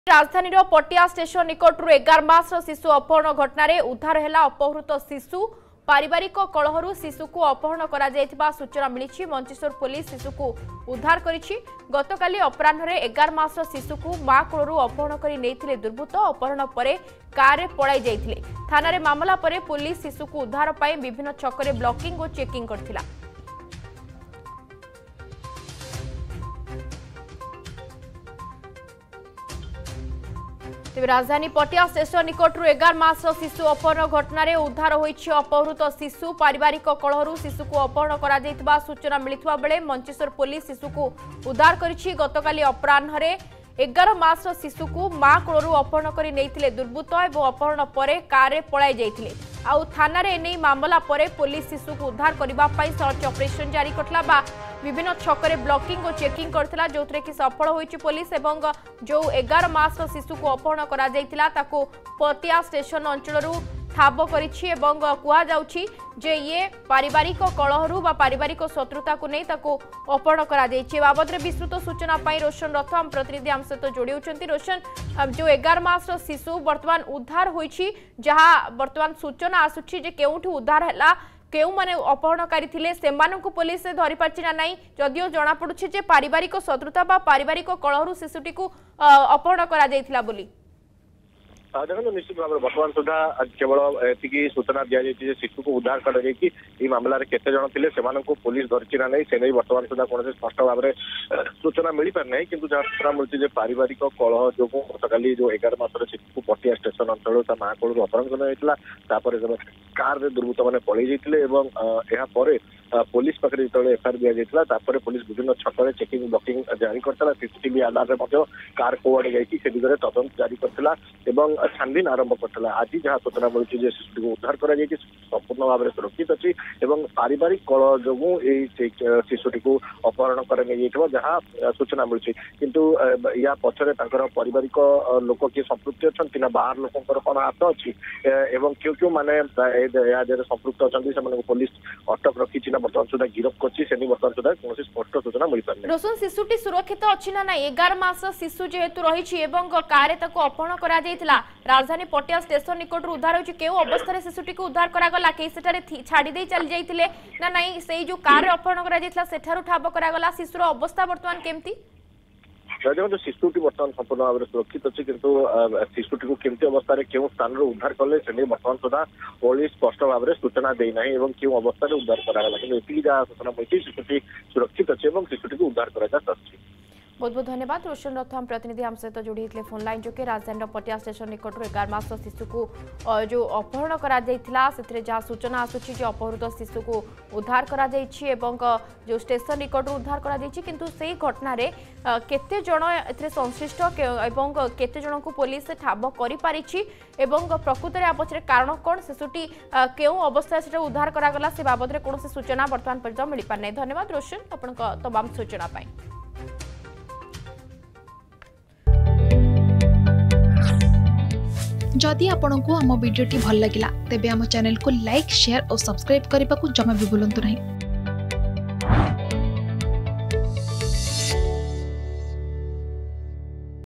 Mancheswar Police Has Rescued The kidnapped 11 Month Old Baby || Child Rescues Bhubaneswar
ରାଜଧାନୀର ପଟିଆ ଷ୍ଟେସନ ନିକଟରୁ 11 ମାସର ଶିଶୁ ଅପହରଣ ଘଟଣା । ଉଦ୍ଧାର ହେଲା ଅପହୃତ ଶିଶୁ । ପାରିବାରିକ କଳହରୁ ଶିଶୁକୁ ଅପହରଣ କରାଯାଇଥିବା ସୂଚନା ମିଳିଛି ।

ରାଜଧାନୀର ପଟିଆ ଷ୍ଟେସନ ନିକଟରୁ 11 ମାସର ଶିଶୁ ଅପହରଣ ଘଟଣା || Child Rescues Bhubaneswar